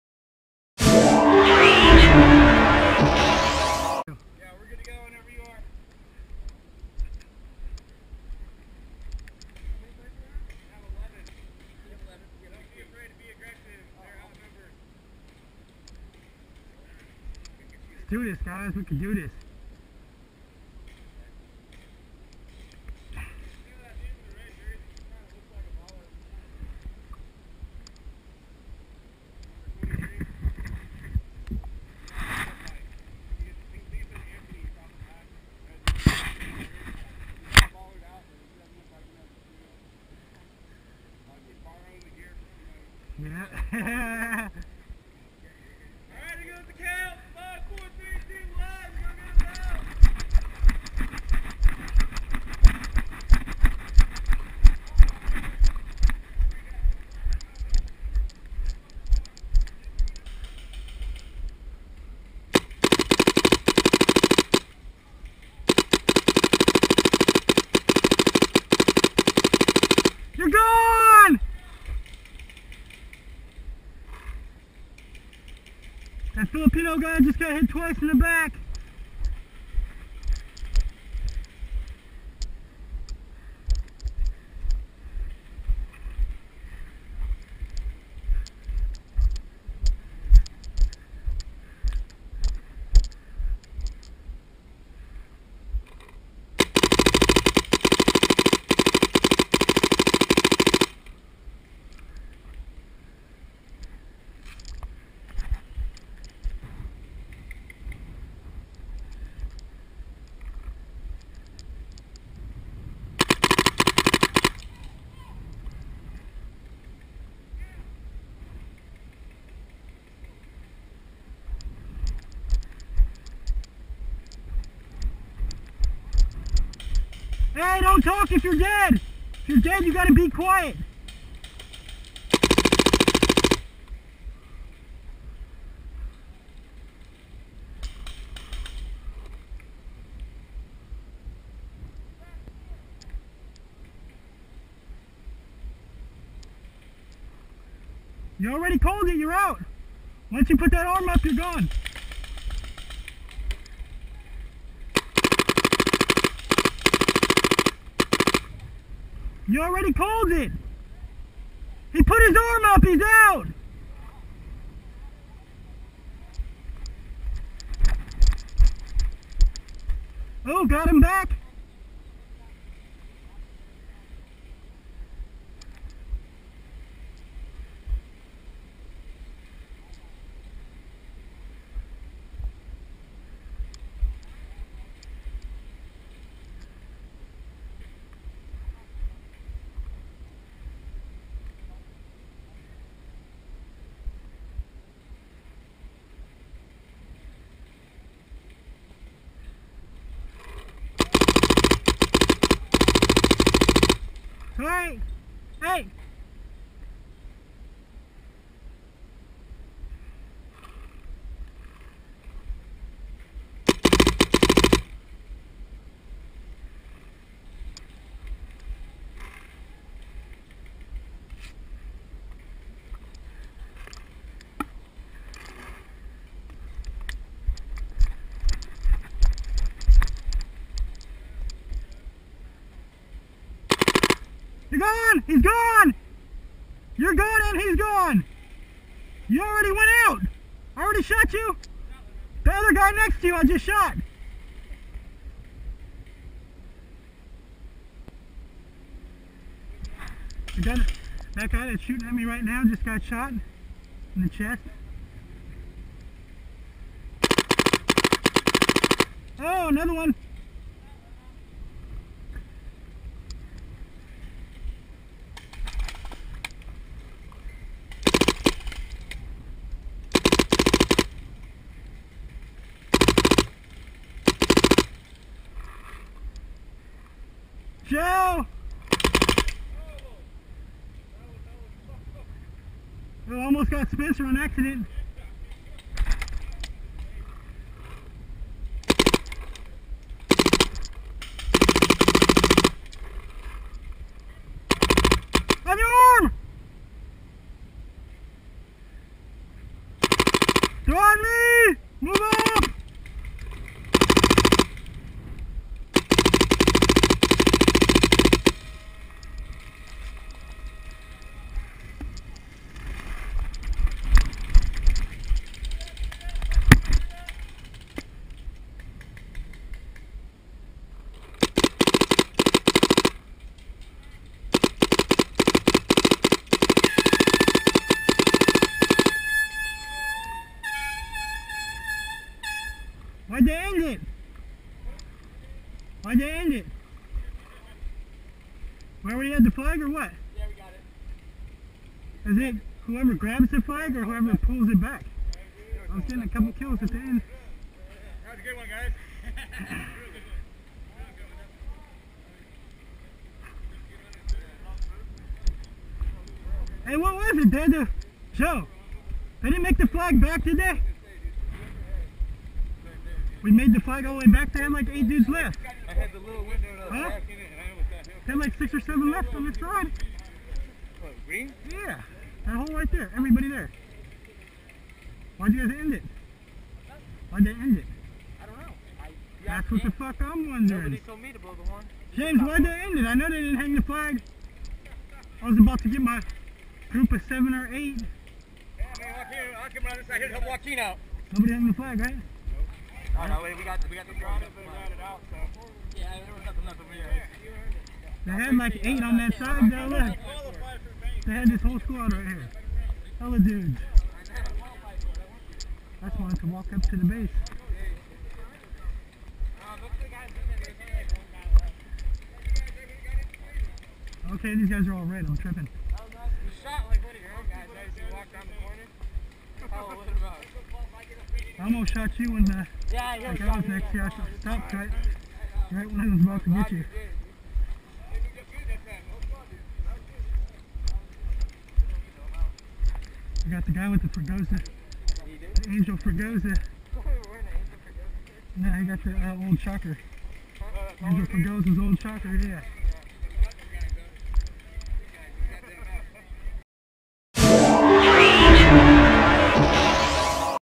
PAINTBALL JUNGLE - JOAQUIN'S BATTLE CAM
here is joaquin using a gopro playing in a flag game.